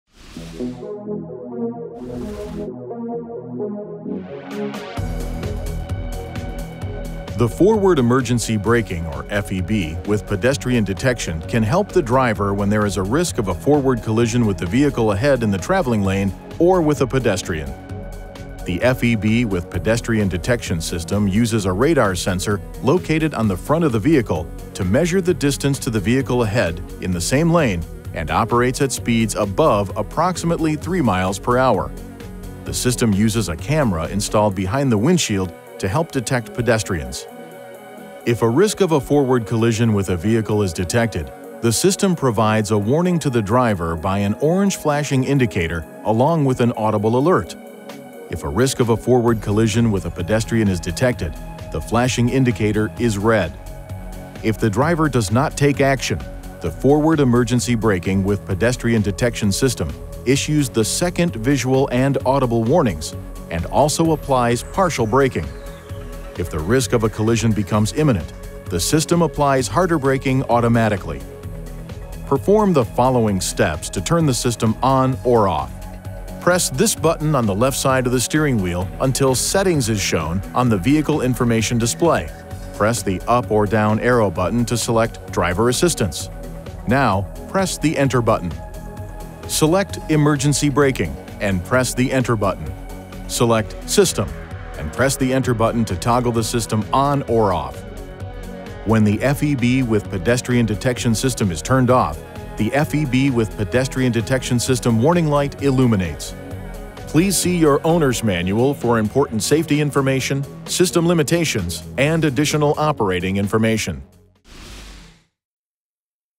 2017 Infiniti QX60 - Forward Emergency Braking (FEB) with Pedestrian Detection System (ise)
The Forward Emergency Braking (FEB) with Pedestrian Detection can help the driver when there is a risk of a forward collision with the vehicle ahead in the traveling lane or with a pedestrian.

The FEB with pedestrian detection system uses a radar sensor located on the front of the vehicle to measure the distance to the vehicle ahead in the same lane, and operates at speeds above approximately 3 miles per hour. The system uses a camera installed behind the windshield to help detect pedestrians.

If a risk of a forward collision with a vehicle is detected, the system provides a warning to the driver by an orange flashing indicator along with an audible alert. If a risk of a forward collision with a pedestrian is detected, the flashing indicator is red.

If the driver does not take action, the Forward Emergency Braking with Pedestrian Detection system issues the second visual and audible warnings and also applies partial braking. If the risk of a collision becomes imminent, the system applies harder braking automatically.

Perform the following steps to turn the system ON or OFF:
● Press this button on the left side of the steering wheel until SETTINGS is shown on the vehicle information display
● Press the up or down arrow button to select “Driver Assistance”
● Now, press the ENTER button.
● Select the “Emergency Braking” and press the ENTER button
● Select “System” and press the ENTER button to toggle the system on or off.

When the FEB with pedestrian detection system is turned off, the FEB with pedestrian detection system warning light illuminates.